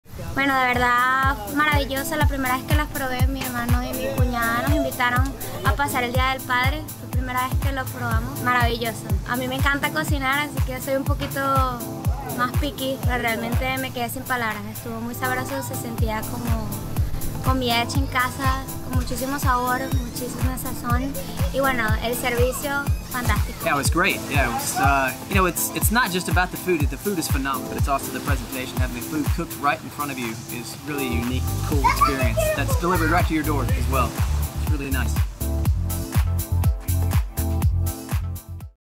Chely y Nathan Oxley cuentan su experiencia con Tasty Paella
Esta maravillosa pareja nos cuentan como fue su experiencia con nuestra compañía, una vez más cumplimos con nuestra misión que es reunir a la familia y inmortalizar esos preciosos ml momentos acompañados de excelente comida española.